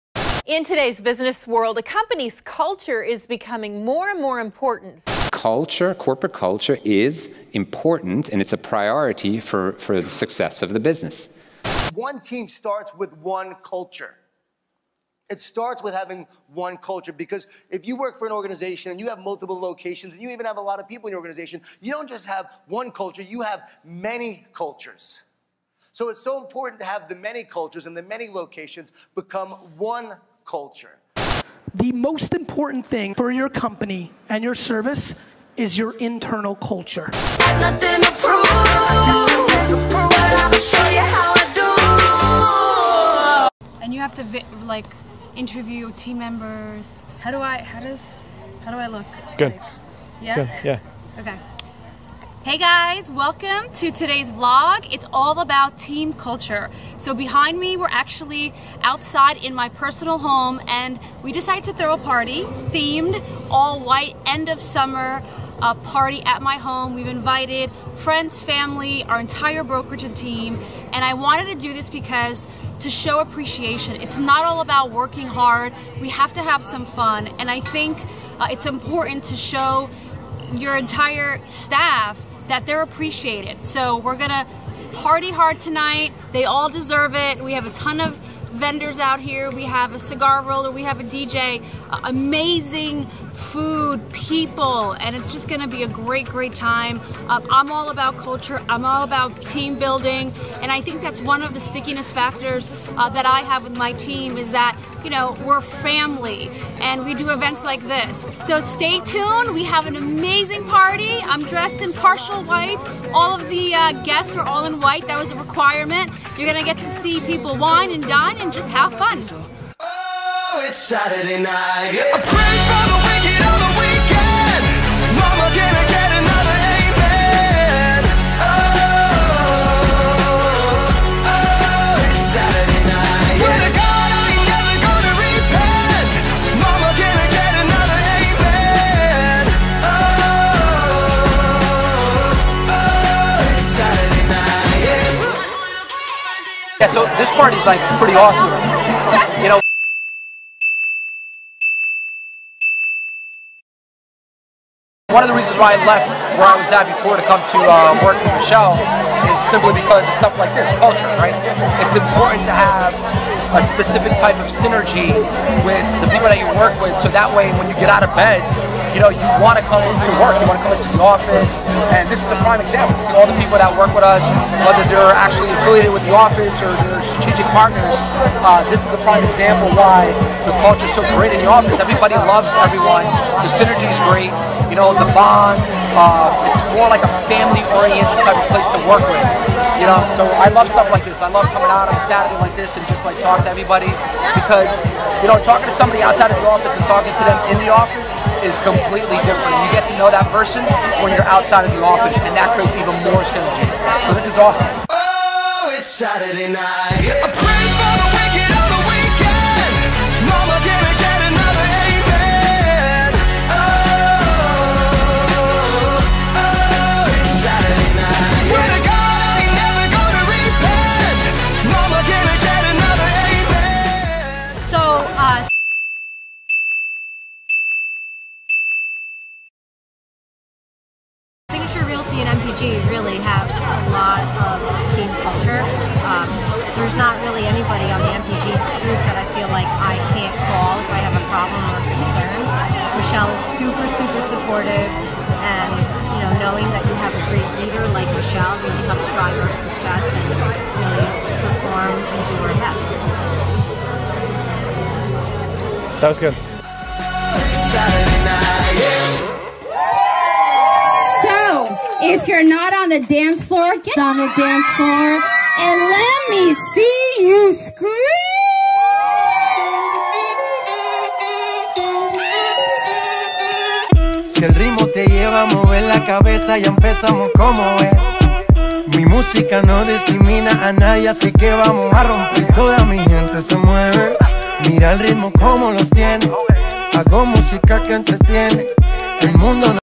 Building Team Culture | Michelle Pais Vlog 04
In this episode, come to Michelle's personal home and hear from her and her team on how they maintain a successful business culture.